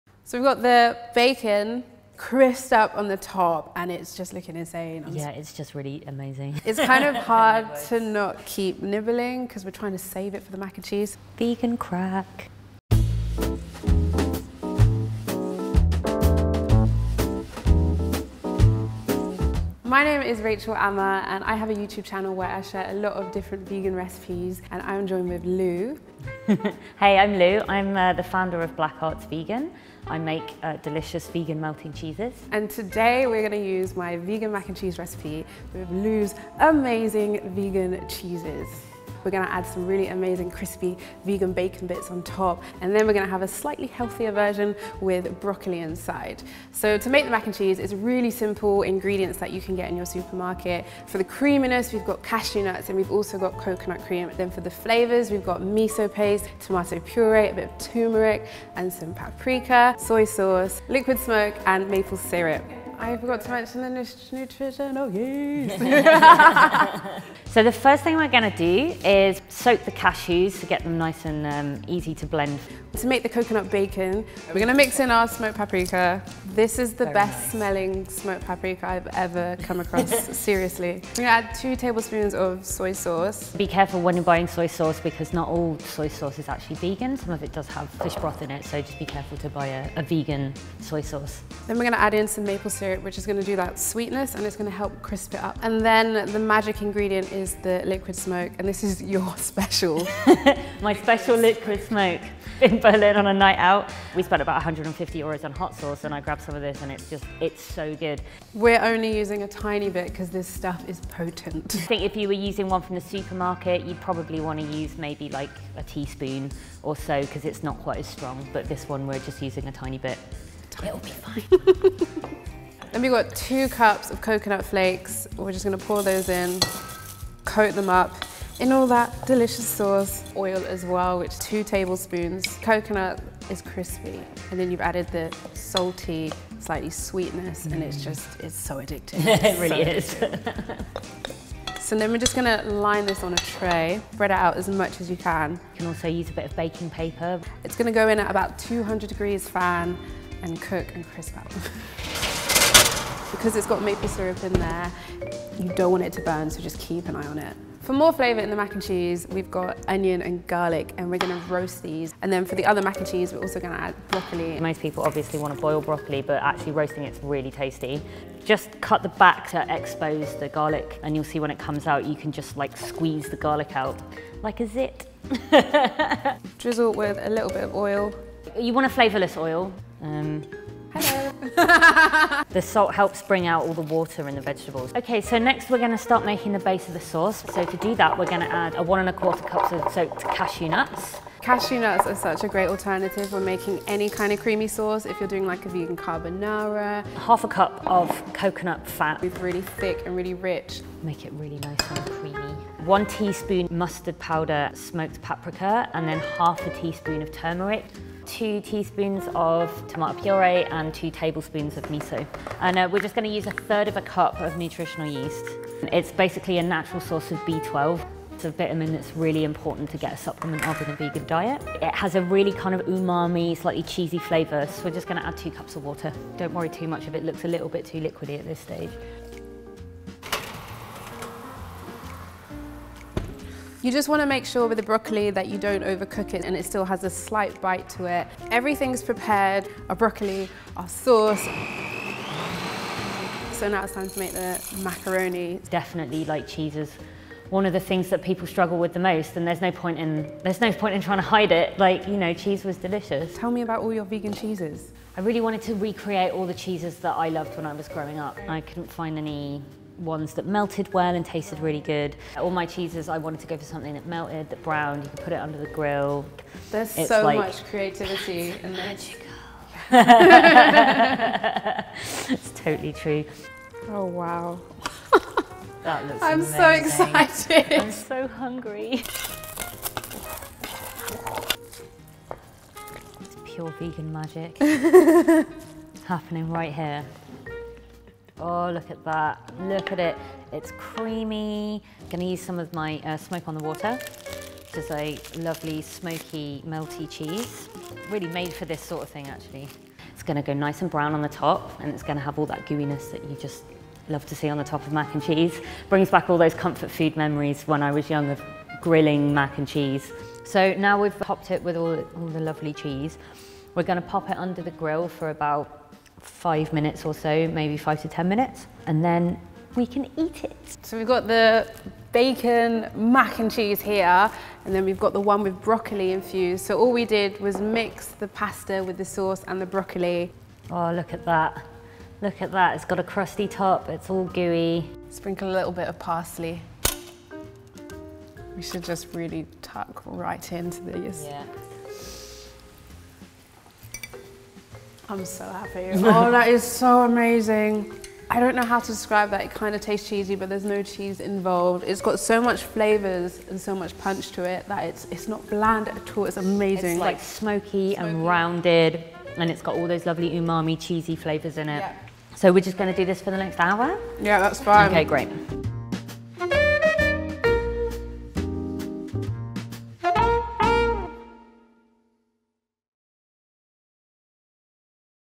How To Make Vegan Mac ‘n’ Cheese with Bacon Bits
Vegan YouTube sensation Rachel Ama and self-described “vegan cheese witch” Lue Cuttiford of Black Arts Vegan show us how to make a creamy, crispy-topped, umami-packed vegan mac ‘n’ cheese—two ways.

One is loaded with roasted broccoli and the other with spoonfuls of totally addictive coconut bacon bits. Because there’s no such thing as too much mac ‘n’ cheese, right?